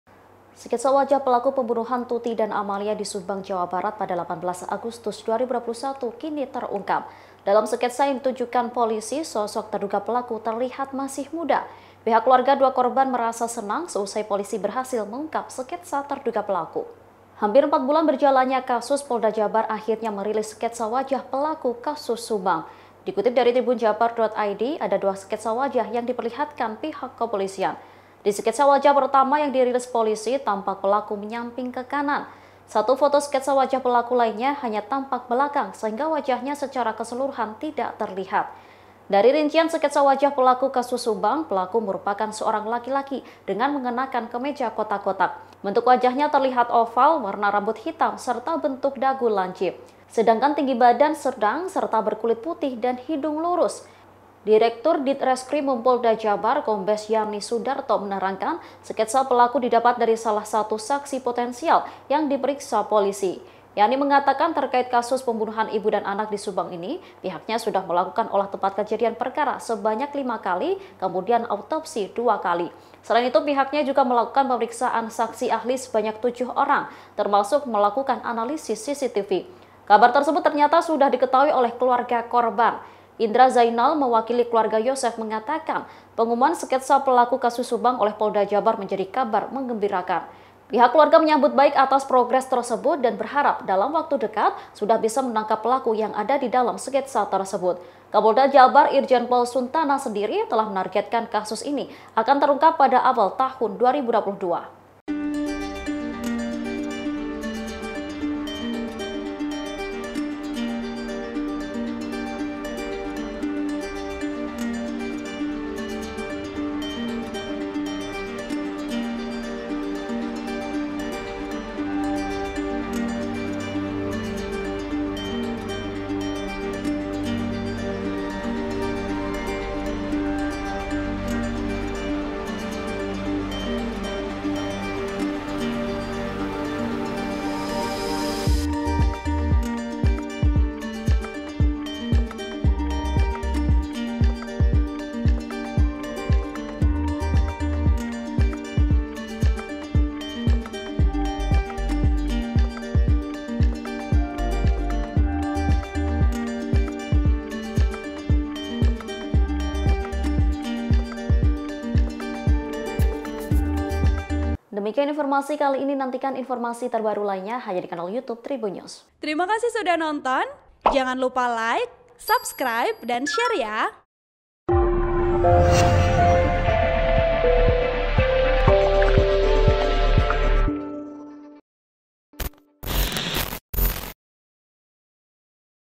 Terungkap Ciri-ciri Terduga Pelaku Pembunuhan Tuti-Amel di Subang, Laki-laki Muda dengan Wajah Oval
TRIBUN-VIDEO.COM - Sketsa wajah pelaku pembunuhan Tuti dan Amalia di Subang pada (18/08/2021) terungkap.

Dalam sketsa yang ditunjukkan polisi, sosok terduga pelaku terlihat masih muda.

Pihak keluarga dua korban merasa senang seusai polisi berhasil mengungkap sketsa terduga pelaku.

Hampir empat bulan berjalannya kasus, Polda Jabar akhirnya merilis sketsa wajah pelaku kasus Subang.